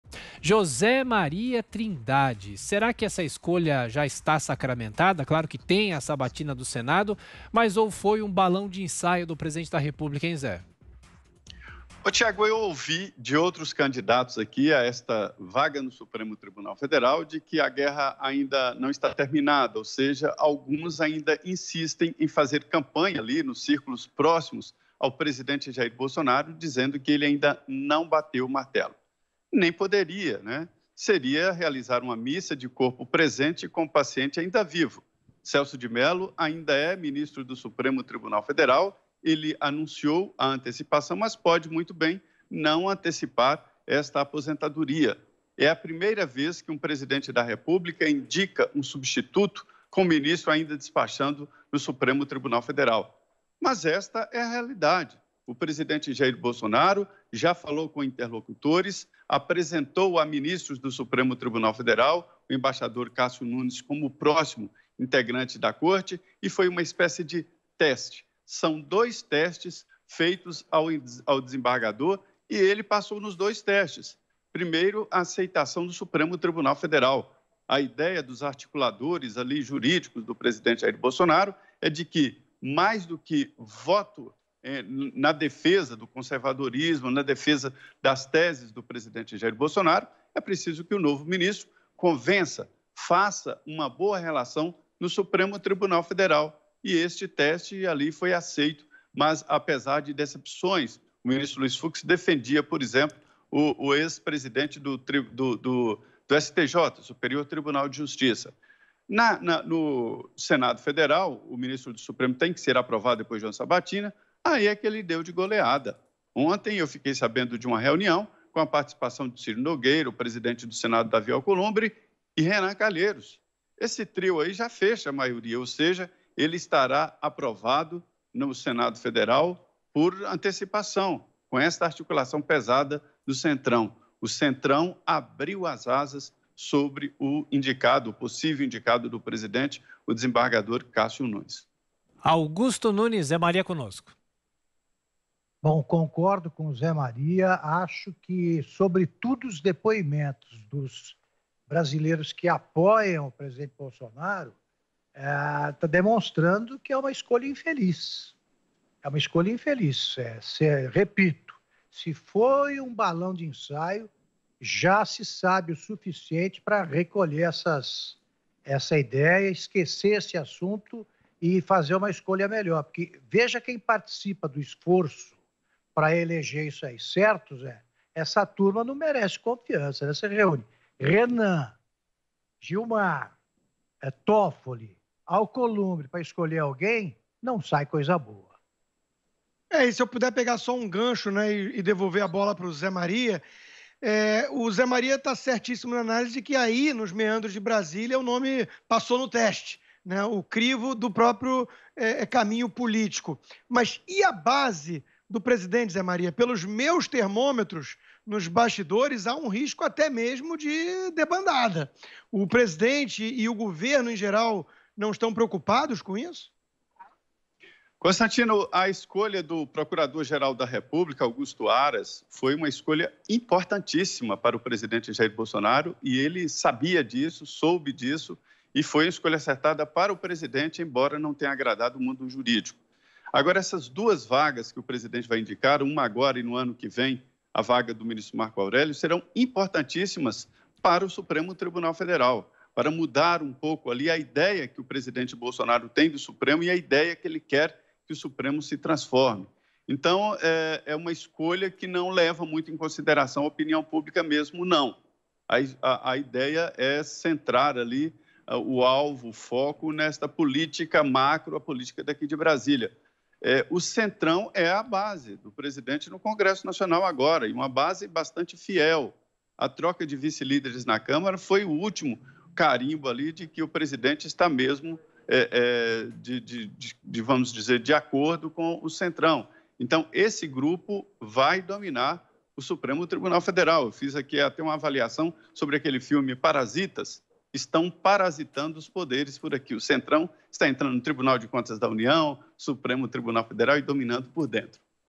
Trindade: "Mello pode não antecipar a aposentadoria"
José Maria Trindade comenta a possível escolha de Bolsonaro para o Supremo Tribunal Federal.